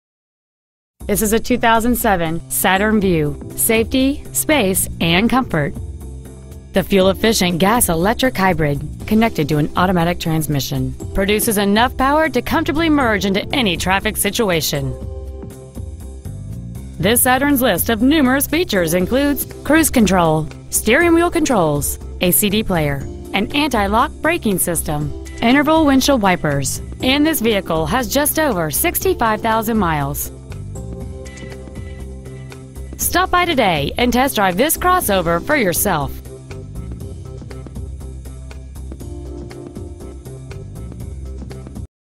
Used 2007 Saturn Vue Chehalis WA
Year: 2007
 Make: Saturn
 Model: Vue
 Engine: 2.4L I4 MFI Hybrid
 Trans.: Automatic
 Exterior: Green
 Miles: 65,918
 Interior: Gray

 Pre-Owned 2007 Saturn Vue Chehalis WA